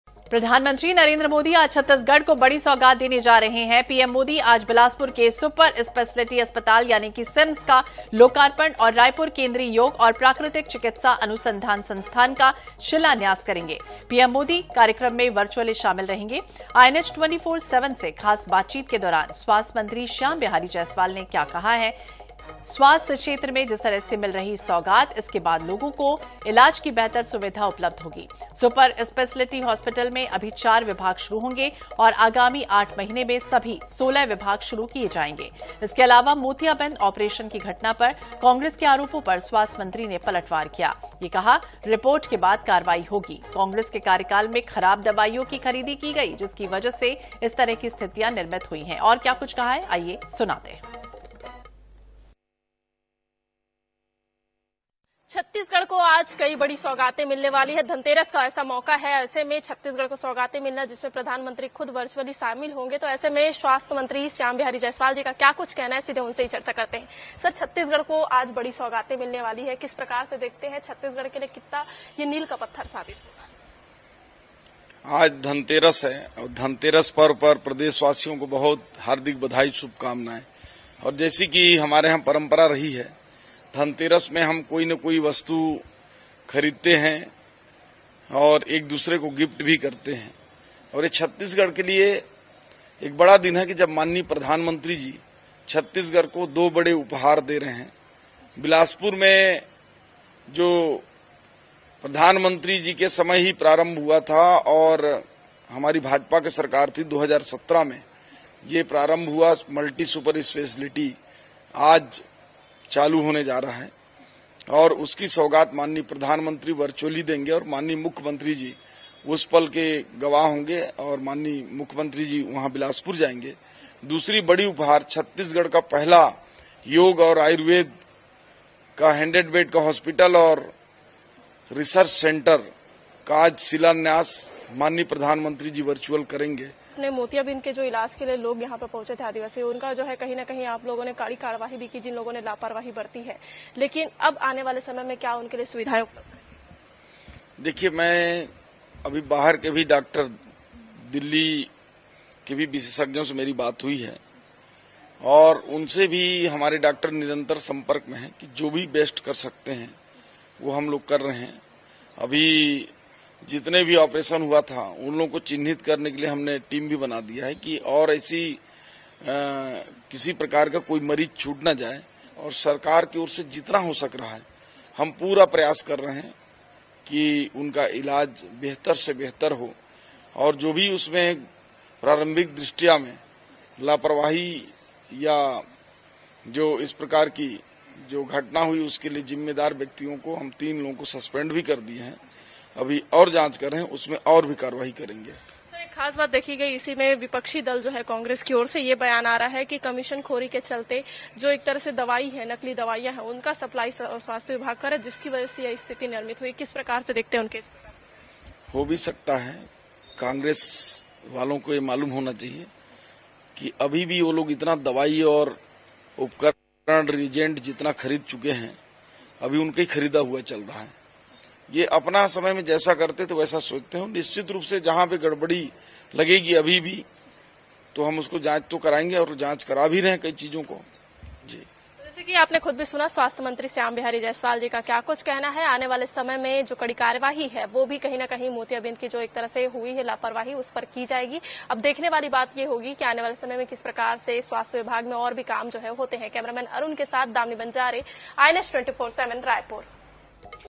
प्रदेश को दो नए अस्पतालों का तोहफा | स्वास्थ्य मंत्री Shyam Bihari Jaiswal से INH 24x7
INH, प्रदेश को दो नए अस्पतालों का तोहफा | स्वास्थ्य मंत्री Shyam Bihari Jaiswal से INH 24x7

Incoming Search Terms: